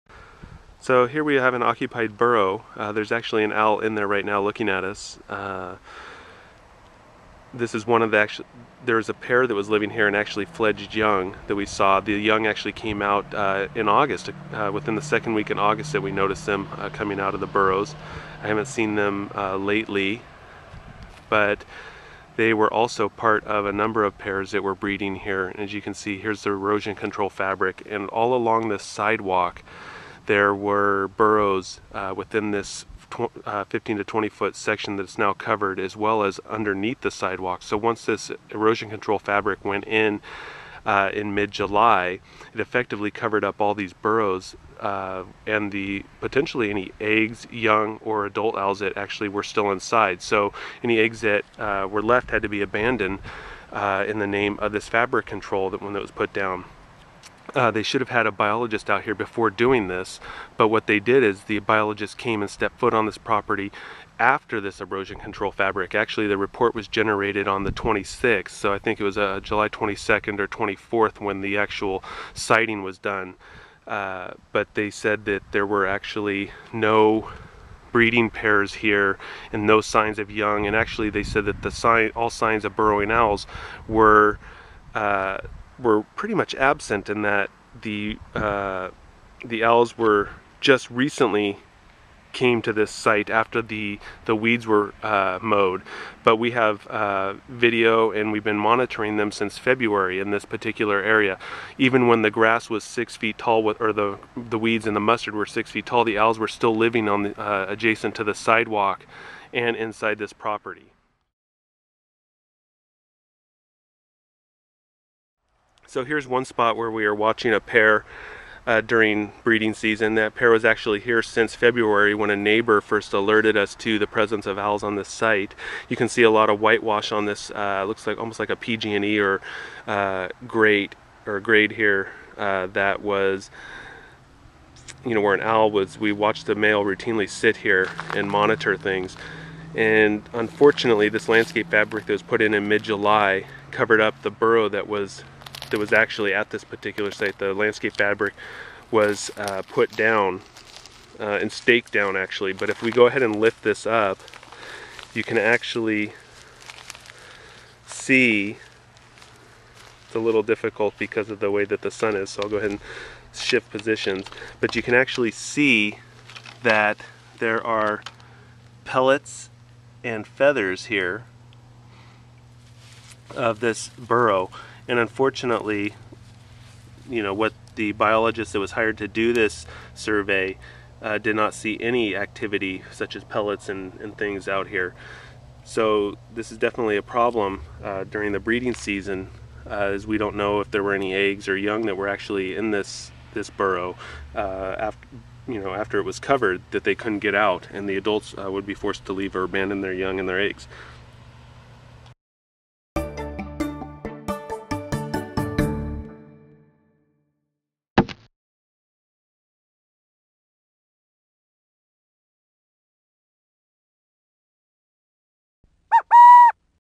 Burrowing Owl Colony Loses Burrows
Burrowing owls are facing sharp declines in California due to habitat loss, lack of education, etc.  In this example we discovered that a developer in Contra Costa County failed to follow proper procedures and covered up the burrows of nesting burrowing owls during the 2010 breeding season. This is not an isolated incident.